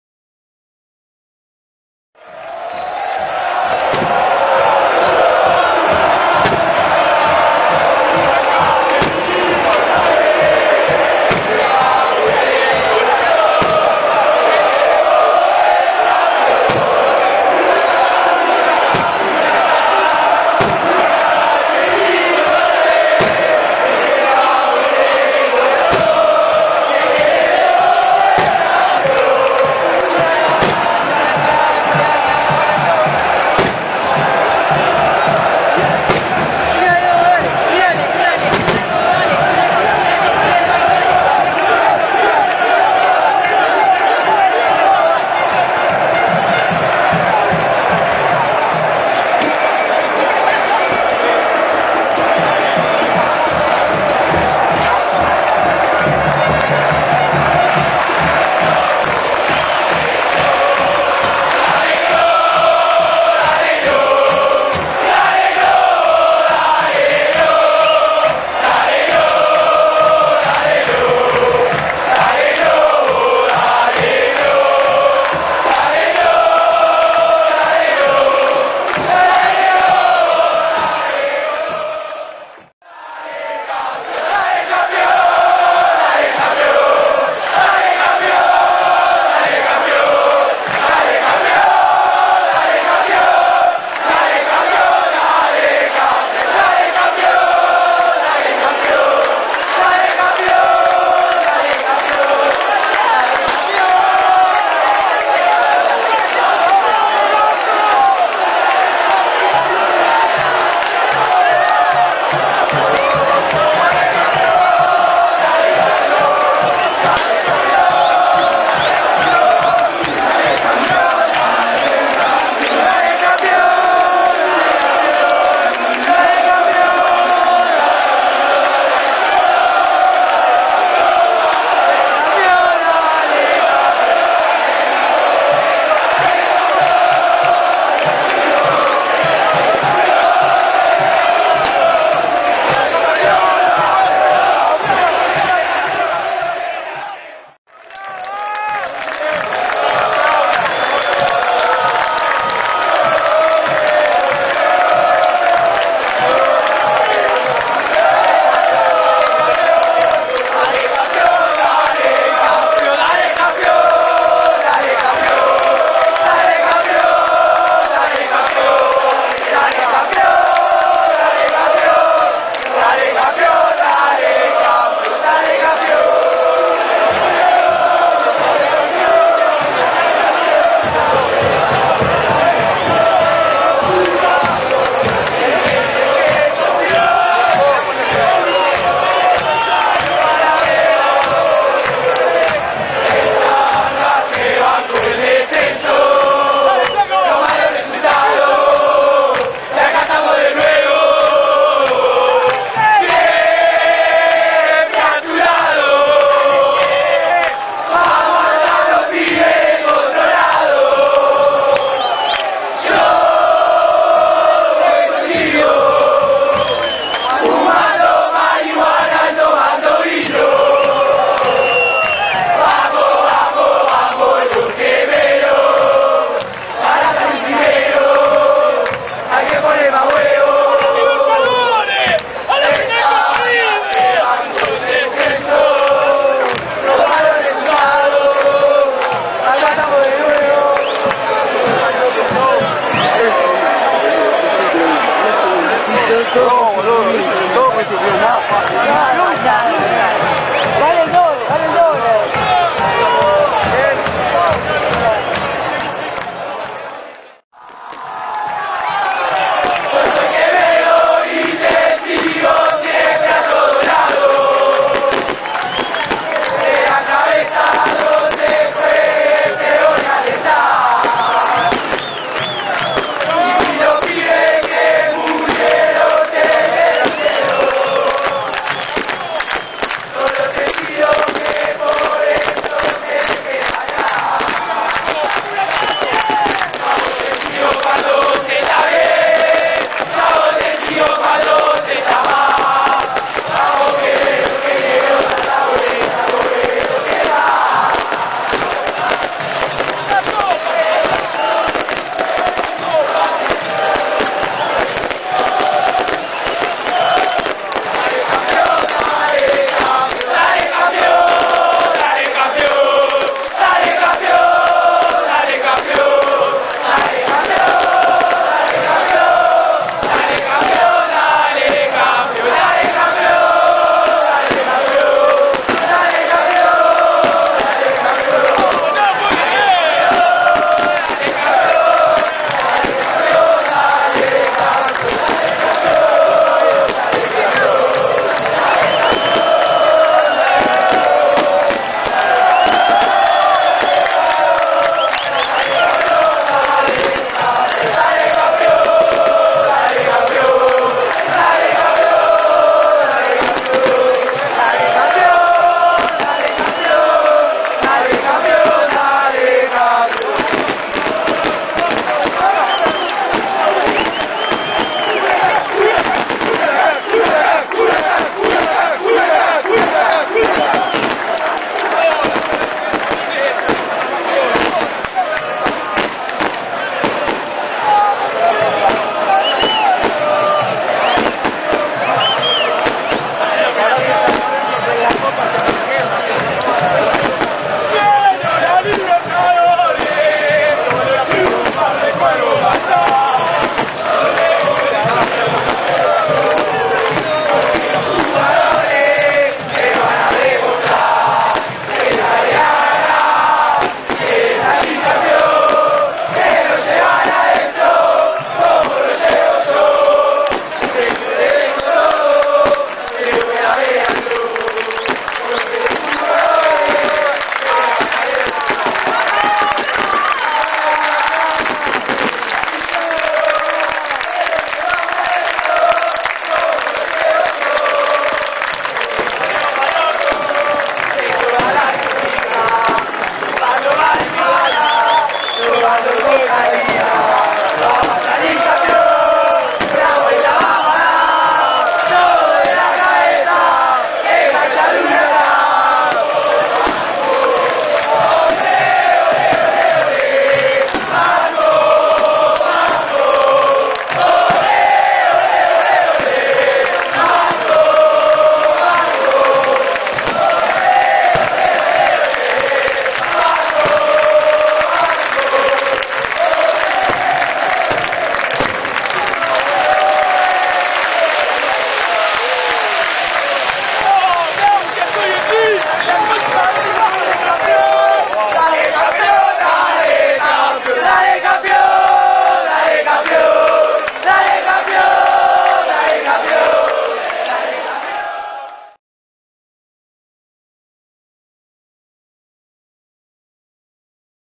Rosario Central 0 vs Huracán 0 (4 a 5 en penales) - Final Copa Argentina
Torneo: Copa Argentina 2014 (Final)
Día: 26 de Noviembre de 2014
Horario: 20.15 Hs.
Estadio:  Estadio del Bicentenario (San Juan)
Árbitro: M. Vigliano
Resultado: 0 - 0 (Víctoria por penales 5 a 4)
Penales: G. Martinez (erró)
E. Dominguez (convirtió)
R. Ábila (erró)
R. Erramuspe (convirtió)
A. Torassa (convirtió)
F. Mancinelli (convirtió)
C. Arano (Convirtió)